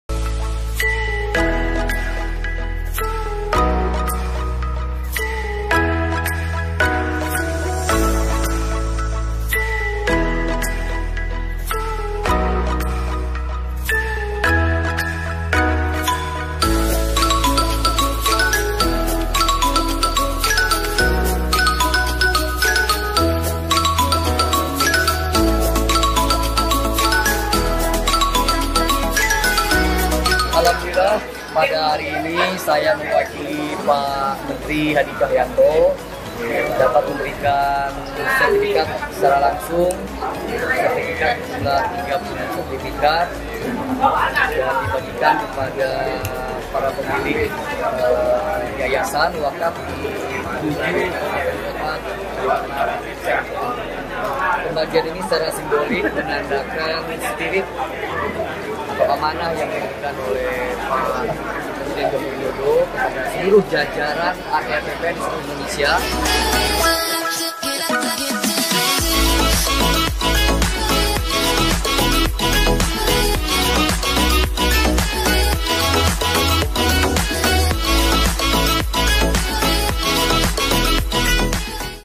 KUNKER WAMEN ATRWAKA BPN DI KALIMANTAN BARAT
Halo
Wakil Menteri ATR/Wakil Kepala BPN, @rajaantoni melaksanakan kunjungan kerja ke Provinsi Kalimantan Barat. Dalam kesempatan ini, Raja Juli menyerahkan 36 sertipikat wakaf.

Simak kegiatan lengkapnya di video berikut ya, Sob! 👆🏻